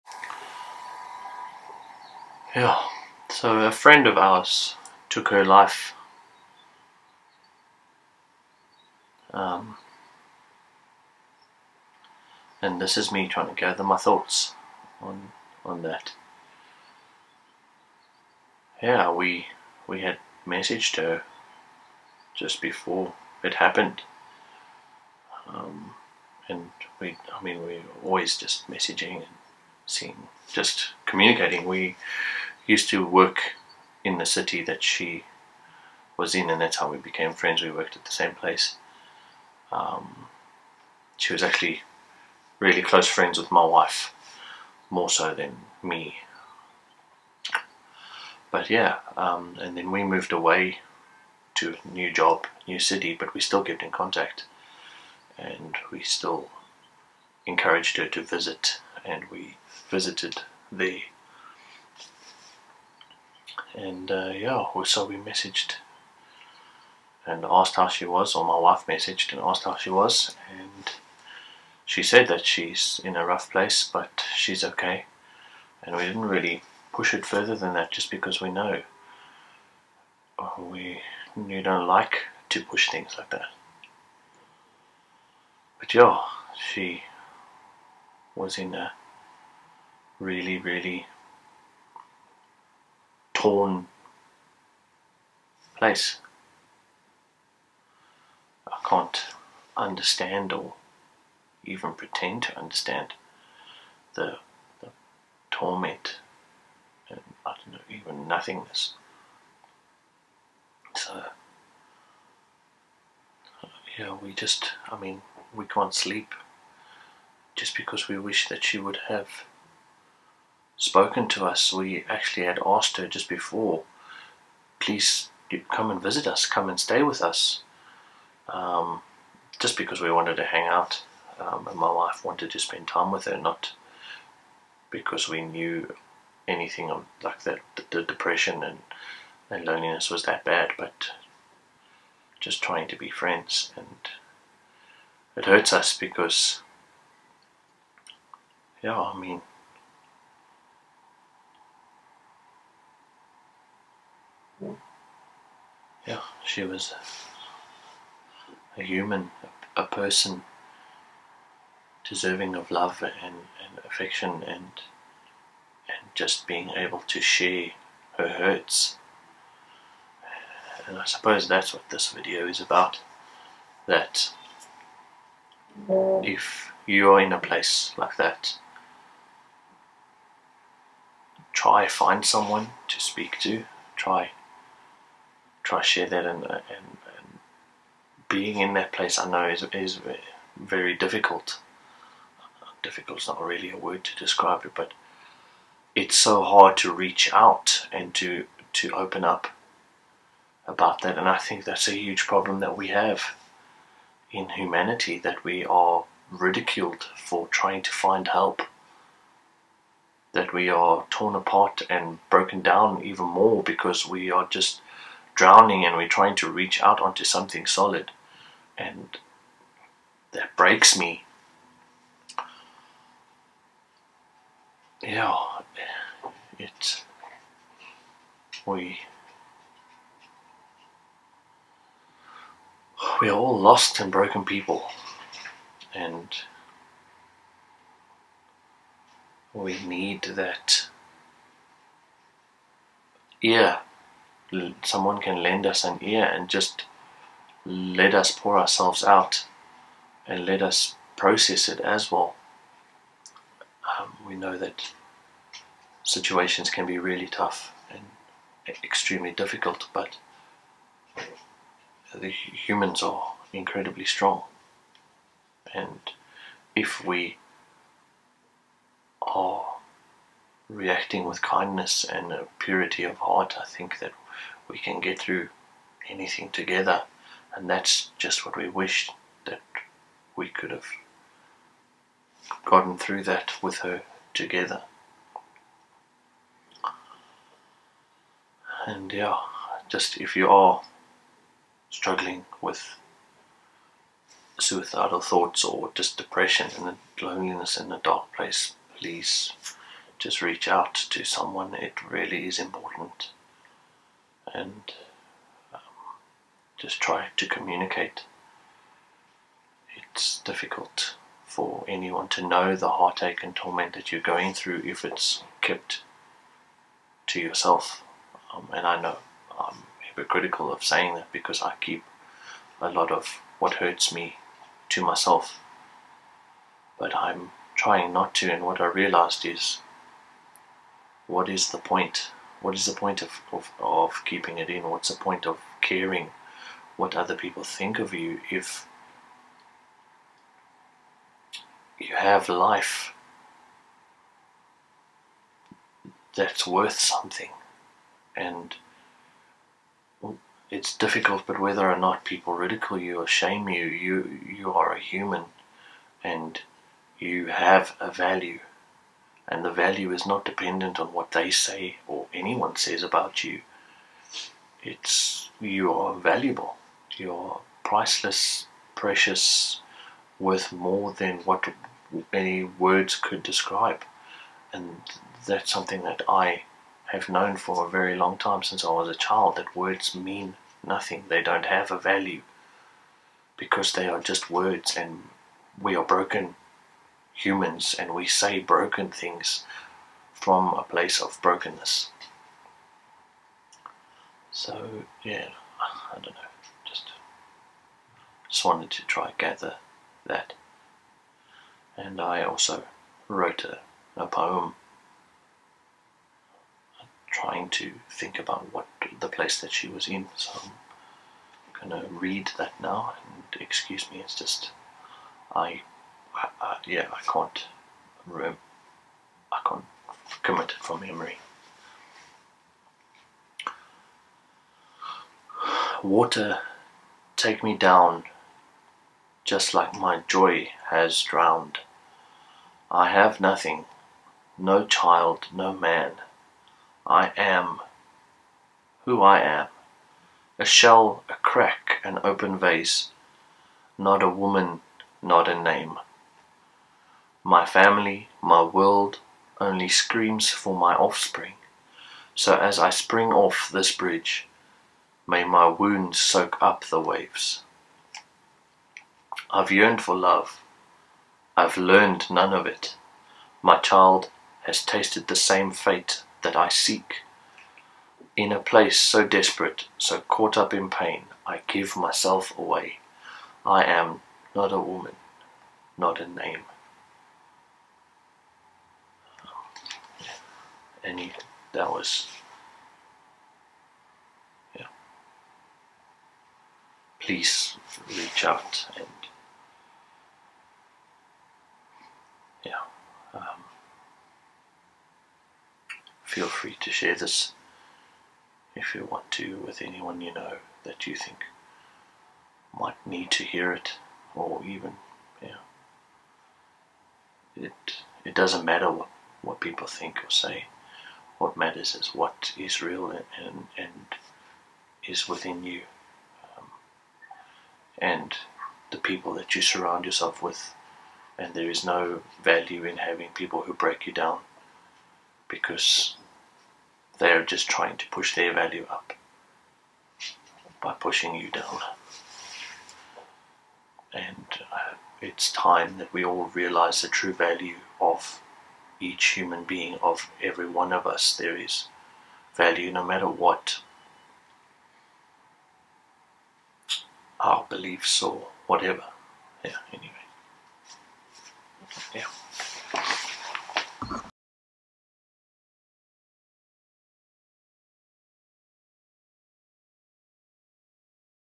"Our Friend Ended It All - But Why?" - A Discussion on Suicide Prevention
A friend of ours had gotten to an exceptionally difficult and lonely place, and obviously (we assume) felt that she couldn't share her hurts with us and her other friends & family. So she decided to run into a river and end her pain.

This is me trying to gather and voice my thoughts and just try, be an encouragement to anyone who feels alone.
It does not matter what others think or say of us, it matters what is within us.
yes it's easier to say that then to actually believed and even more difficult to live it out.
This is also a challenge to every human who watches this to take other peoples emotional, physical and mental well-being into account with every interaction we have. We are a lot of broken people on this earth just trying to pick up the pieces of our lives.

I also share a poem that I wrote from the perspective of what I feel she must of been thinking.

Please contact a professional and pursue professional help and speak to someone close to you or someone you trust, if you feel like you don't have anyone, please contact your nearest church.
( They can and will help. I know from experience.)